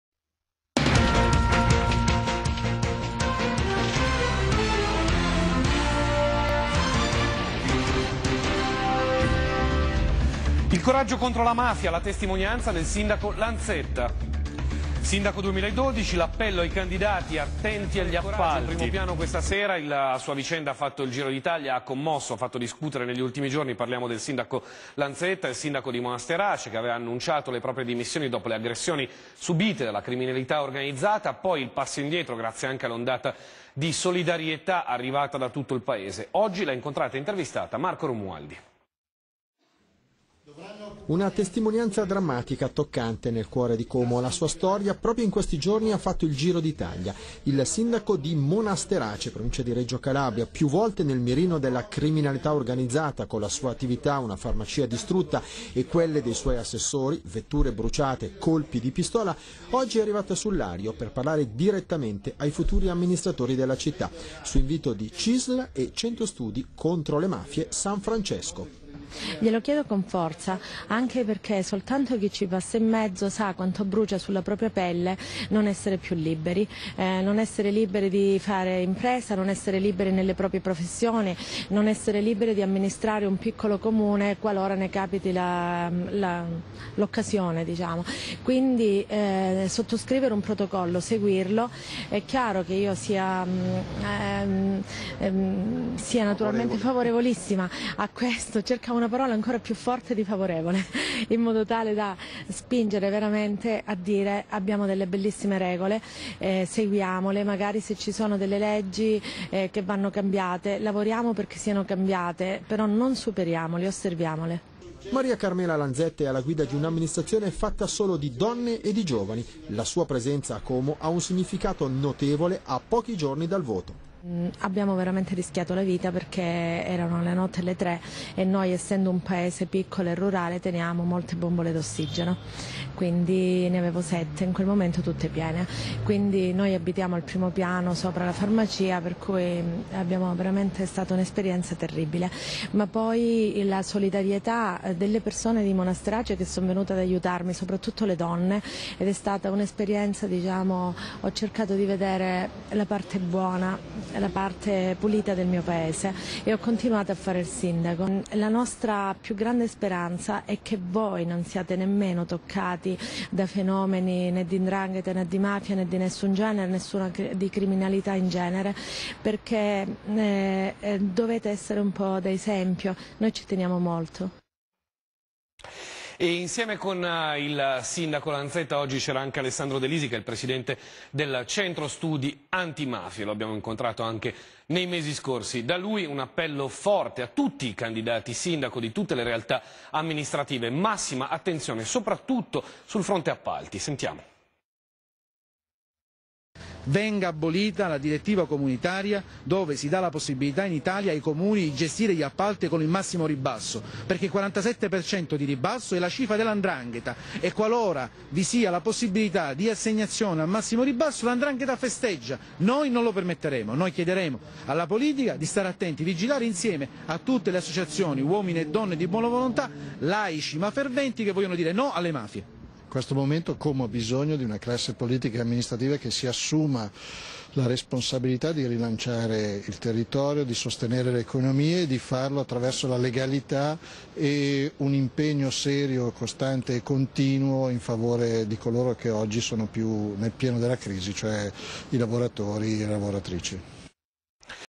Sindaco antimafia con Cisl e Progetto San Francesco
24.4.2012 - Servizio TG di Espansione TV - Maria Carmela Lanzetta, sindaco di Monasterace (RC) oggetto di  minacce di morte da parte della 'ndrangheta è intervenuta all'incontro con i candidati sindaco.promosso da Cisl di Como e Centro Studi Sociali contro le mafie "Progetto San Francesco".